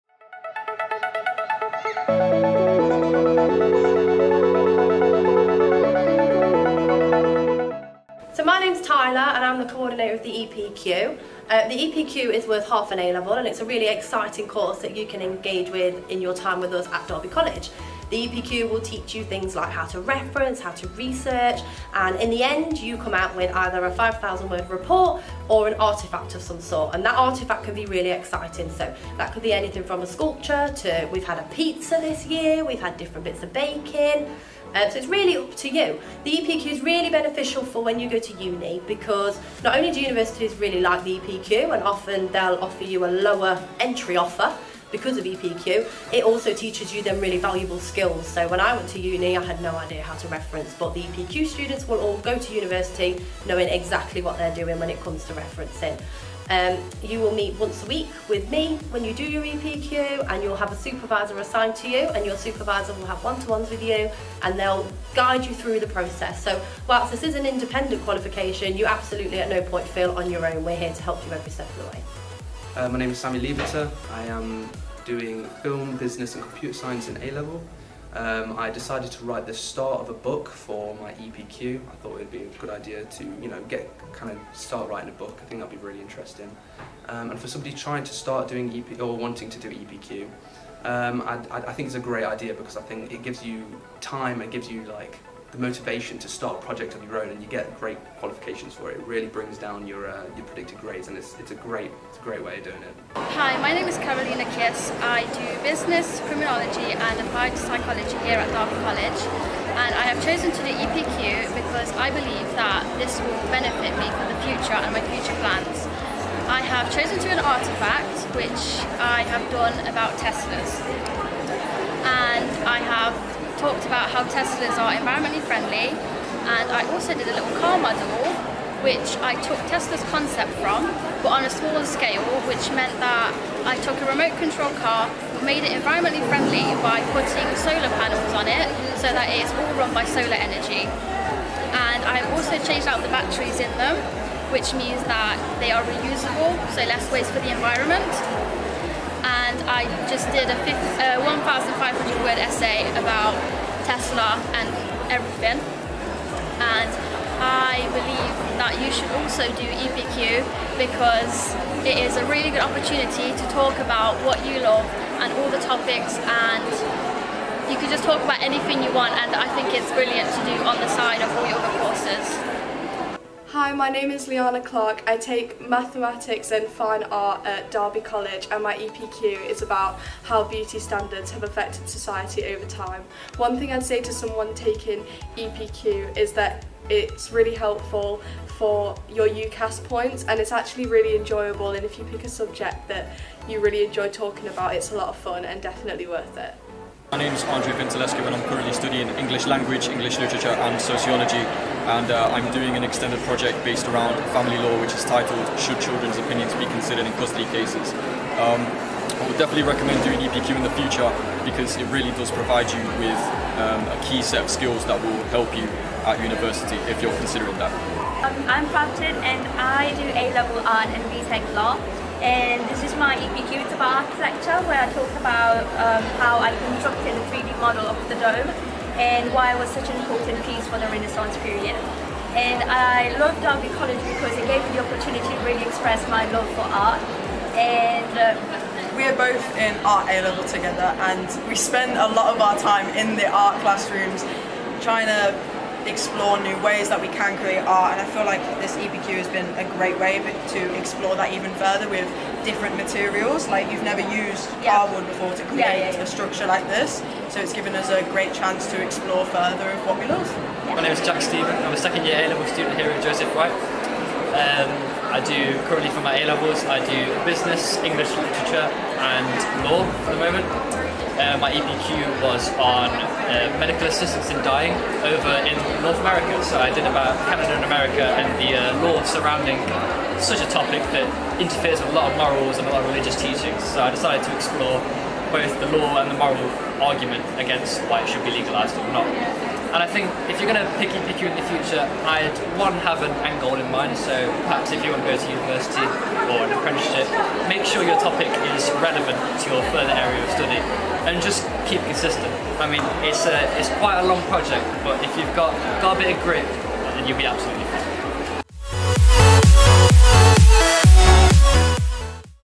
DCG's EPQ Showcase 2023
Extended Project Qualification students showcased their work to over 100 people at the Joseph Wright Centre.